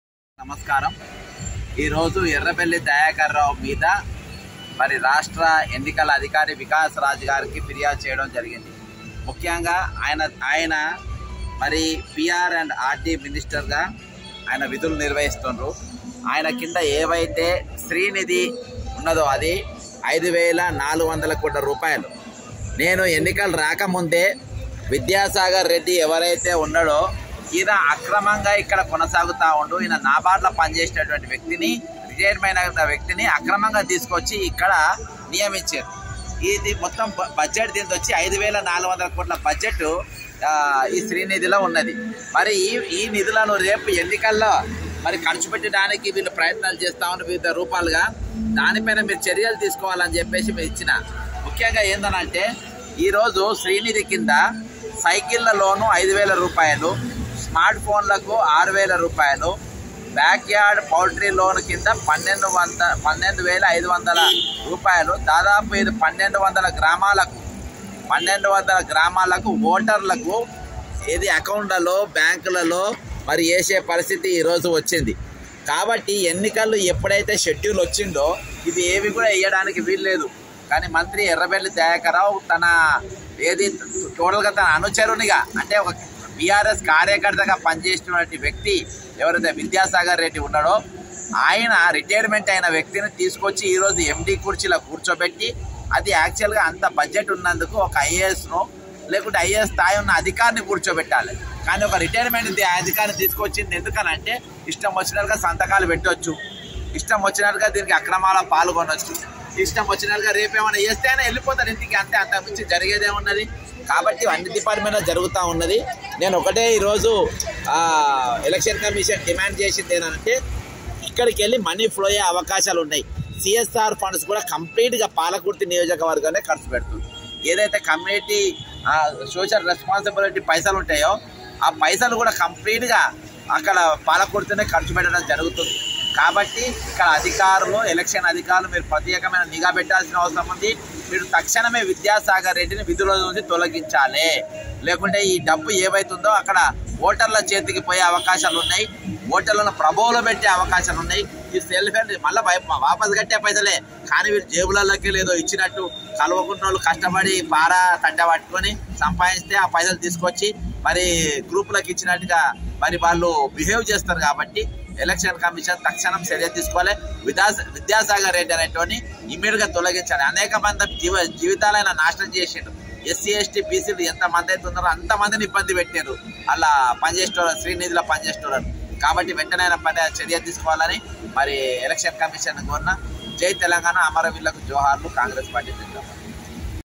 విద్యాసాగర్ రెడ్డి,ఎర్రబెల్లి దయాకర్ 5వేలకోట్లు మిస్ యూజ్ చేస్తున్నారు అని ఎన్నికల కమిషన్..
Video from Anaposala Venkatesham